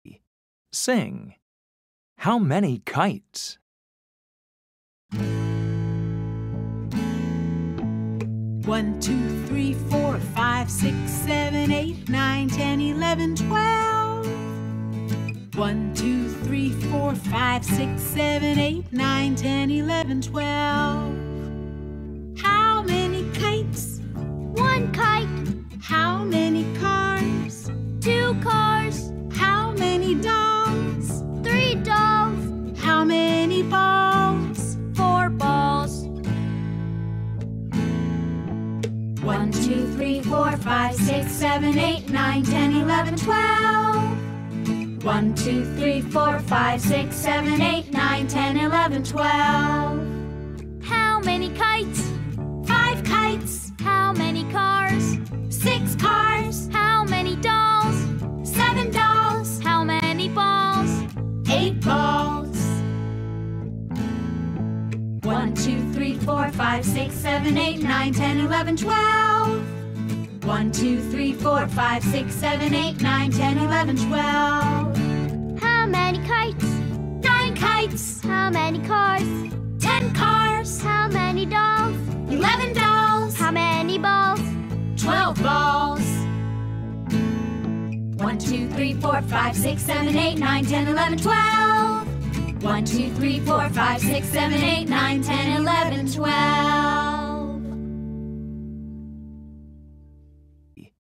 Unit 3.2 _How many kites? Numbers and Toys
How many...?
kites, cars, dolls, and ballls

numbers
1, 2, 3, 4, 5, 6, 7, 8, 9, 10, 11, 12